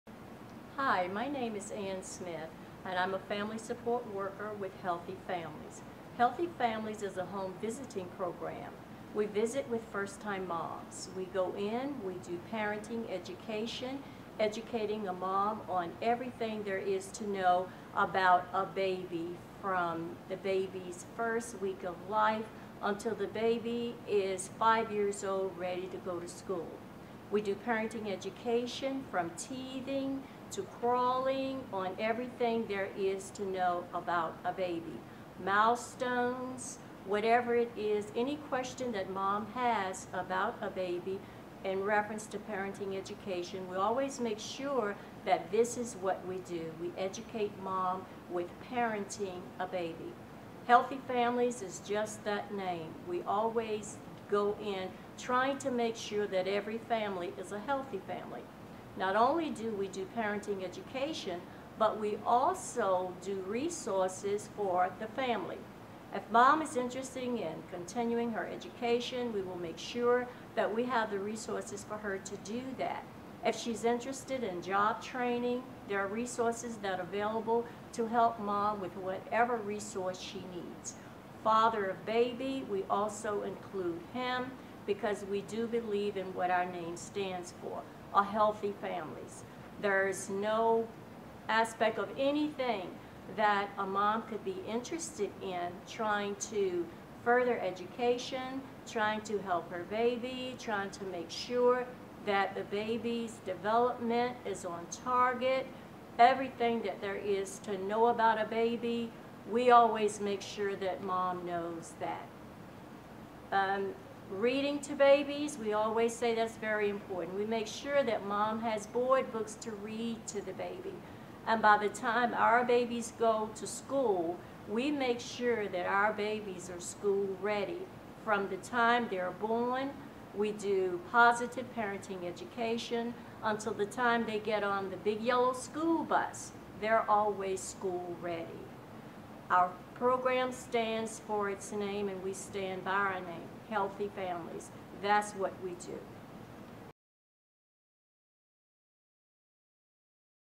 Ann Smith (Family Support Worker with Healthy Families Program)
Here We Grow Suffolk is dedicated to fostering healthy pregnancies and healthy babies for parents scheduled to deliver at Sentara Obici Hospital.  It's a quick reference tool to access through your phone or online.

Here We Grow Suffolk is a collaborative undertaking between Sentara Obici Hospital and the Western Tidewater Health Department to support a healthy, thriving pregnancy and baby.